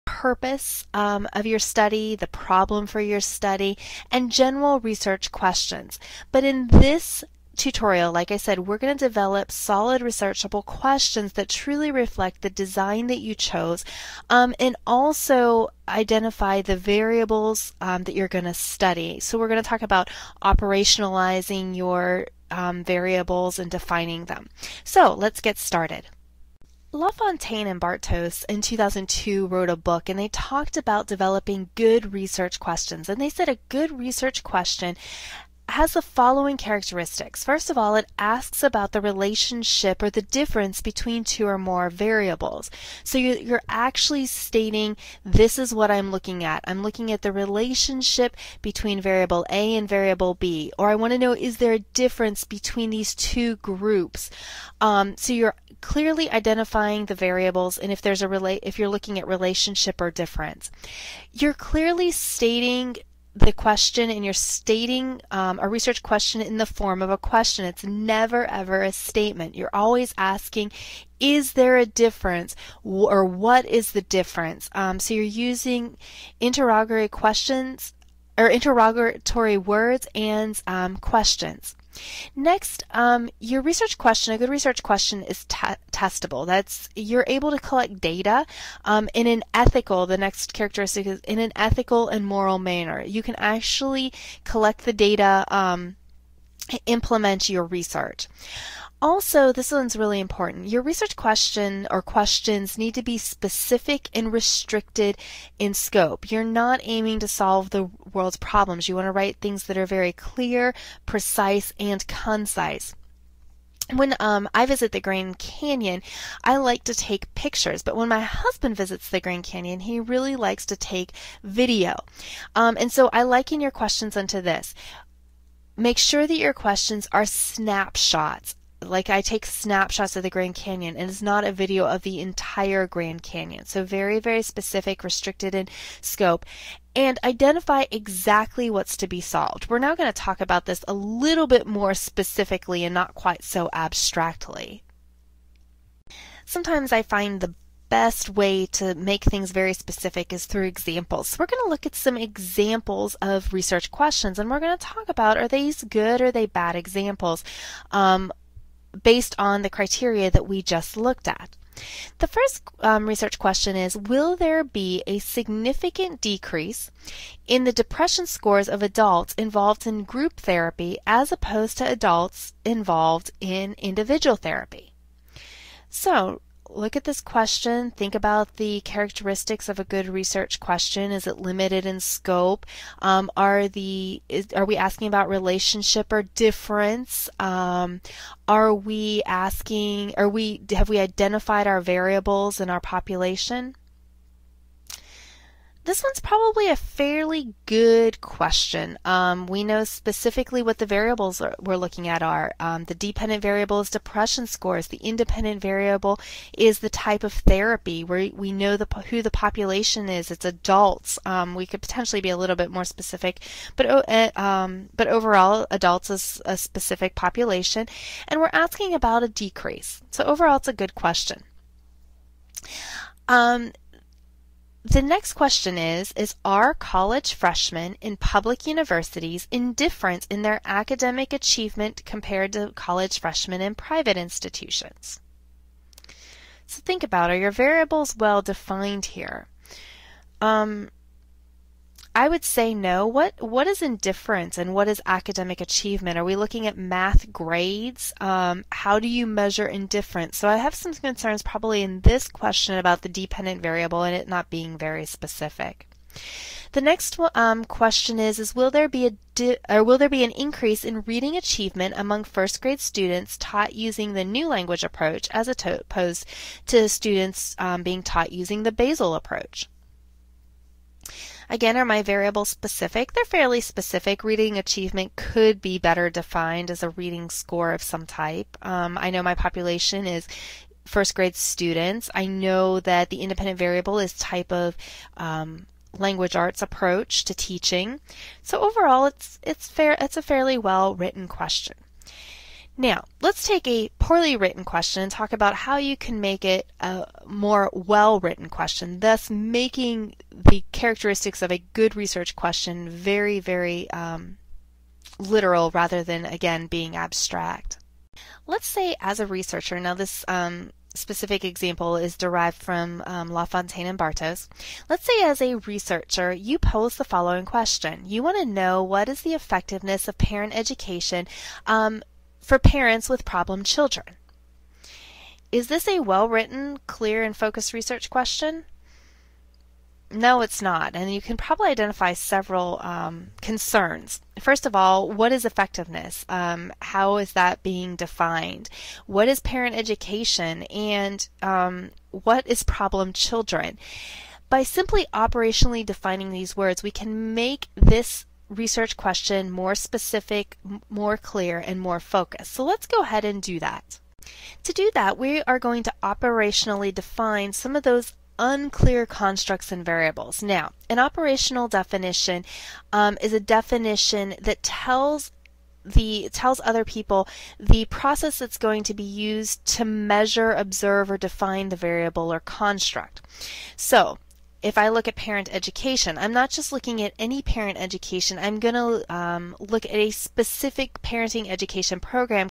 Developing a Quantitative Research Plan: Research Questions
This tutorial focuses on the development of research questions.